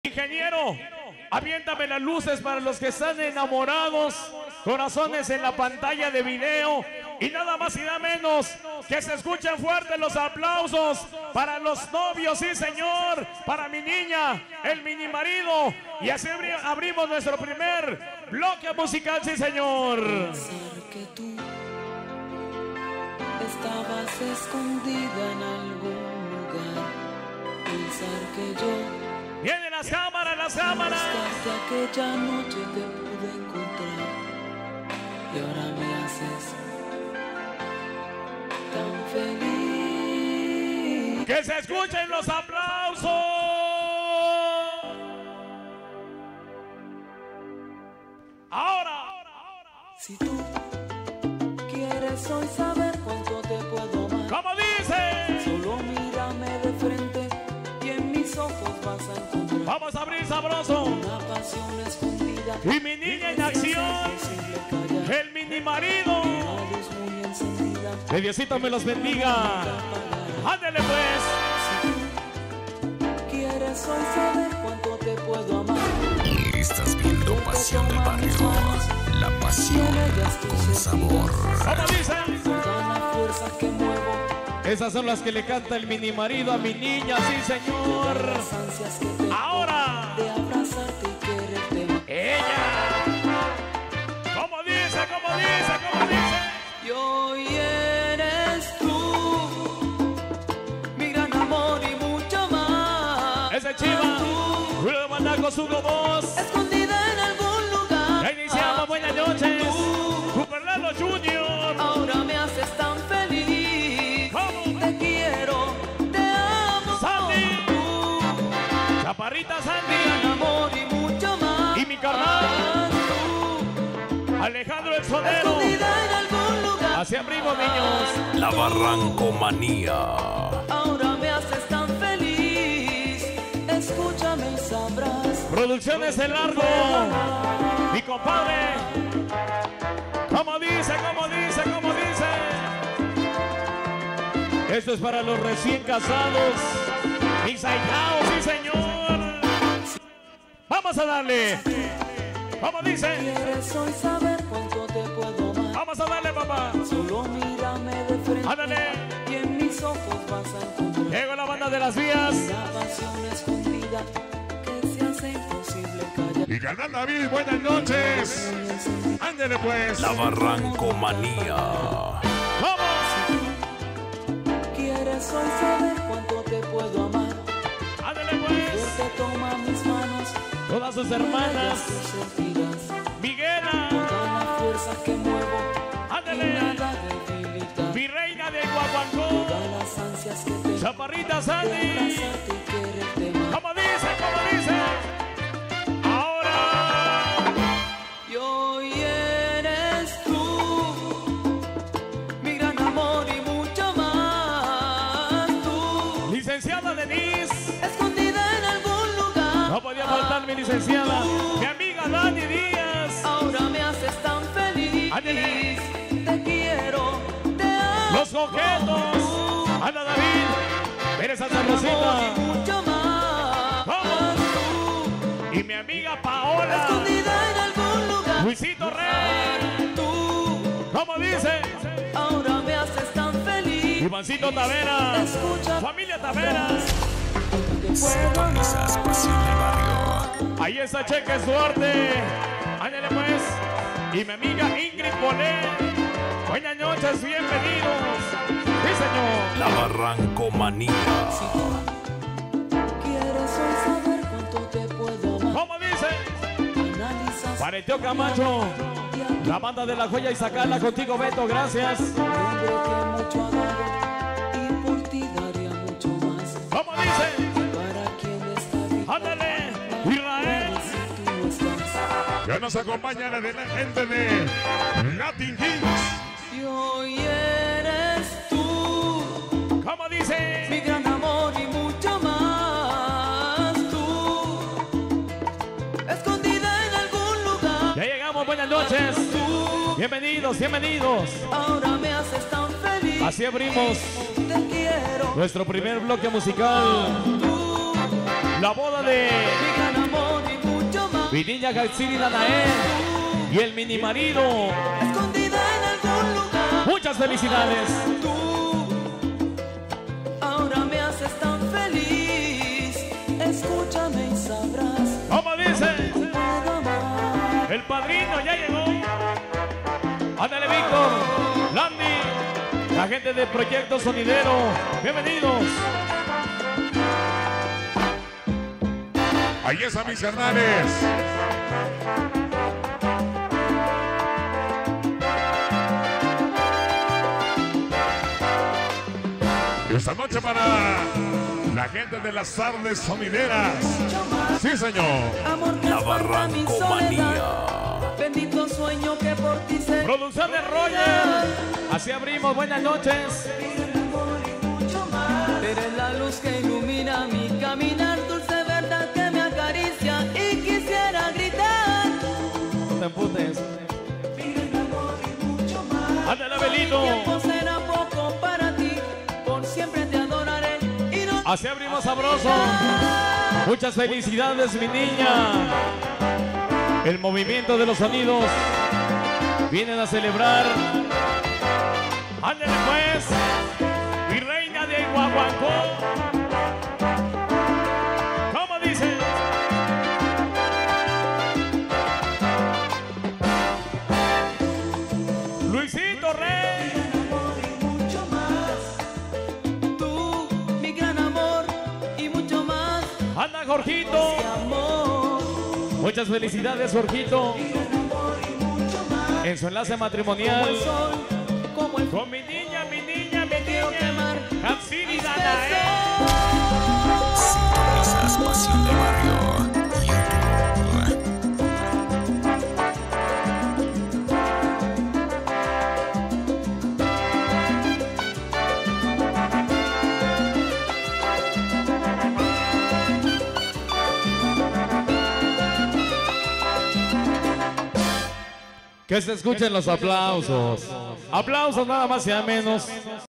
Boda, La Niña y Mini Marido - SENSACION BARRANCO - 25-05-24
una de las fiestas más esperadas de este 2024  la boda de mi niña

COMPARTE

COMENTARIOS